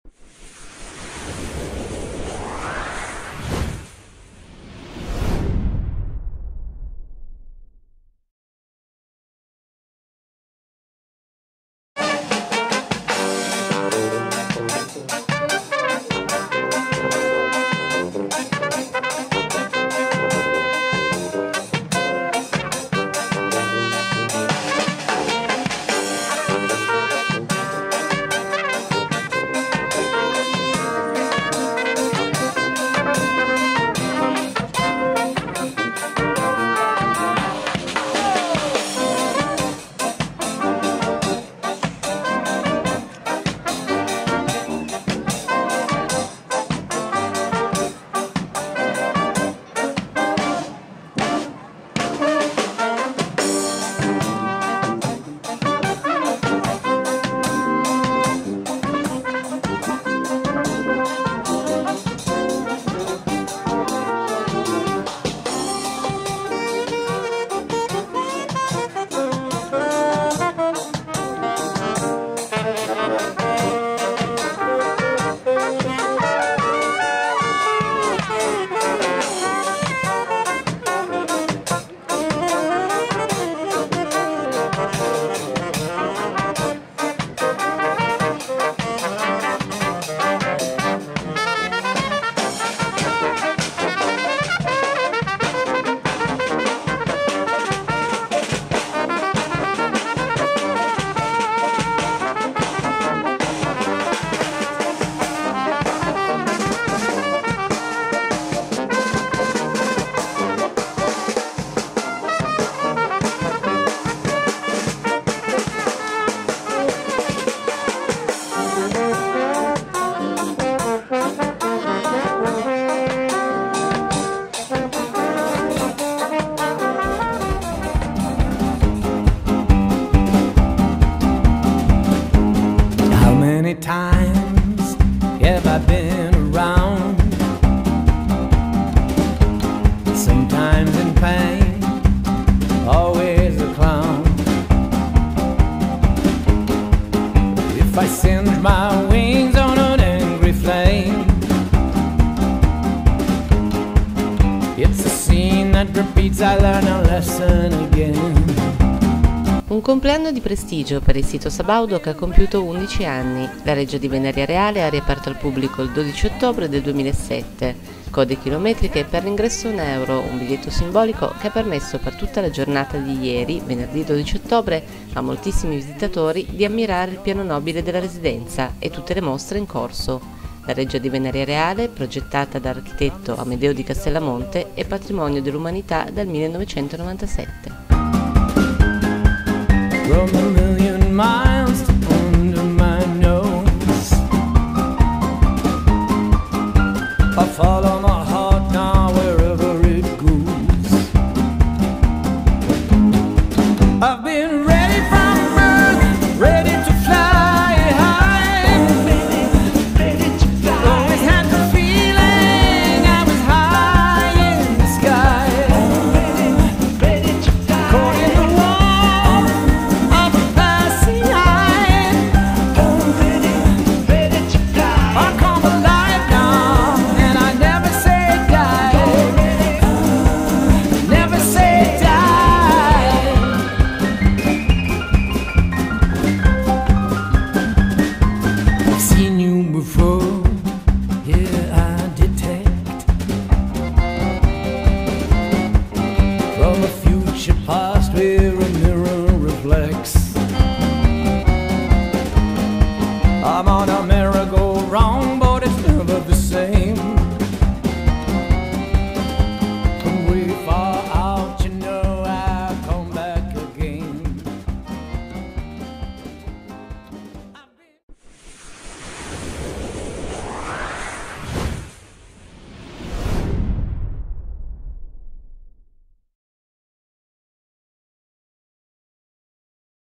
Venaria Reale - La Reggia compie 11 anni
Venaria Reale - La Reggia compie 11 anni: code lunghissime per entrare con il biglietto a 1 euro